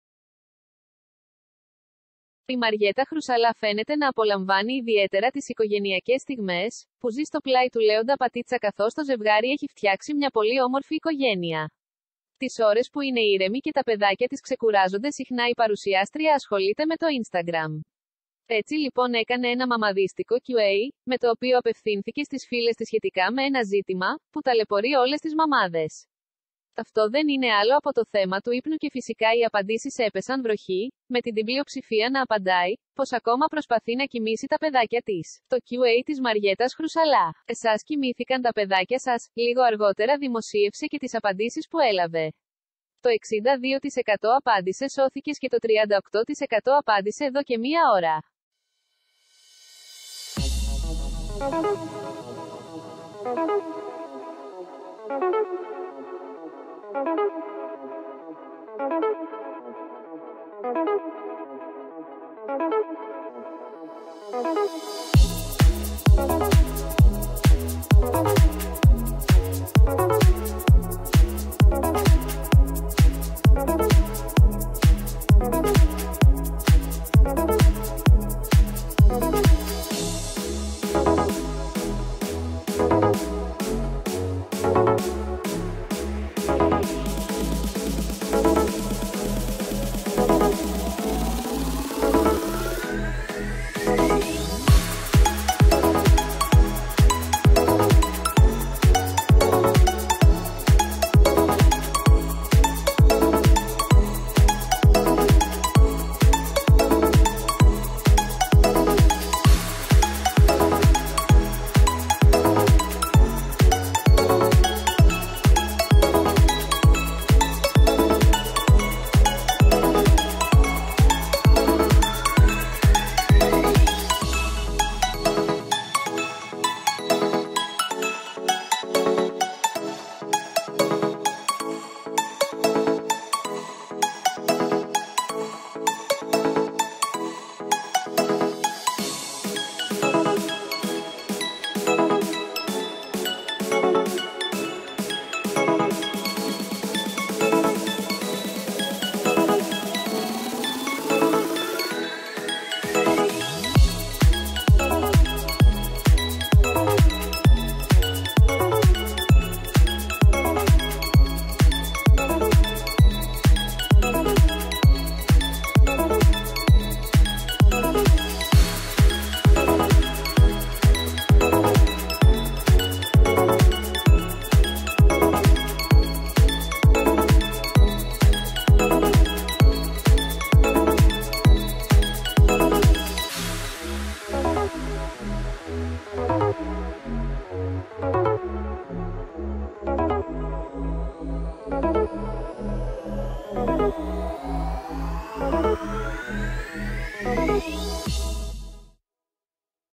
Το αγανακτισμένο Q&A της Μαριέττας Χρουσαλά - «Εσάς κοιμήθηκαν;»
Η Μαριέττα Χρουσαλά φαίνεται να απολαμβάνει ιδιαίτερα τις οικογενειακές στιγμές, που ζει στο πλάι του Λέοντα Πατίτσα καθώς το ζευγάρι έχει φτιάξει μια πολύ όμορφη οικογένεια. Τις ώρες που είναι ήρεμη και τα παιδάκια της ξεκουράζονται συχνά η παρουσιάστρια ασχολείται με το Ιnstagram.